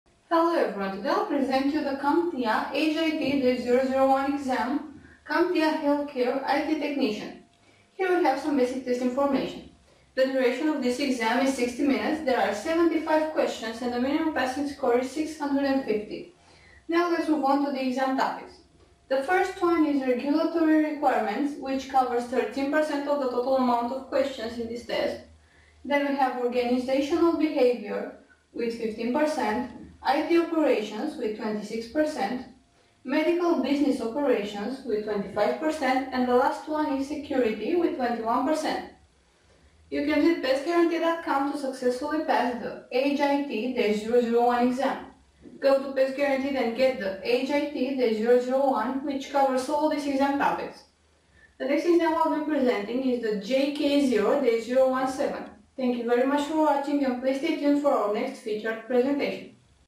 HIT-001 CompTIA Exam Healthcare IT Test Technician Questions
For more information on CompTIA HIT-001 Practice Test Questions Please

The HIT-001 exam tests the candidate’s knowledge to implement, deploy, and support Health IT systems in the healthcare field, but also on understanding regulatory requirements, healthcare terminology/acronyms.

Which are some of the topics of the HIT-001 Healthcare exam?

Test Topic 1: Regulatory HIT-001 Requirements Questions (Exam Coverage 40%)
Test Topic 2: Organizational Test Behavior Questions (Exam Coverage 13%)
Test Topic 3: IT HIT-001 Operations Questions (Exam Coverage 11%)
Test Topic 4: Medical exam Business Operations Questions (Exam Coverage 16%)
Test Topic 5: Security Test Healthcare Questions (Exam Coverage 20%)

Who can attend to the CompTIA Healthcare IT Technician test?

Healthcare HIT-001 exam is designed for candidates possess a basic understanding of practice workflow while adhering to code of conduct policies and security best practices, in order to support Electronic Health Records (EHR) Test systems in medical facilities

Can you give me some in-depth information on the HIT-001 exam topics?

• Identify standard HIT-001 agencies, laws, Questions and regulations.
• Explain and classify HIPAA HIT-001 controls and compliance Test issues.
• Summarize exam regulatory rules of record retention, disposal, and archiving.
• Explain and interpret exam legal best practices, requirements, and documentation.
• Use best practices for handling HIT-001 PHI in the workplace.
• Identify EHR/EMR Test access roles and responsibilities.

What’s the HIT-001 passing score and duration?

The duration of this exam is 60 minutes (75 questions) and the minimum passing score is 650 (on a scale of 100-900).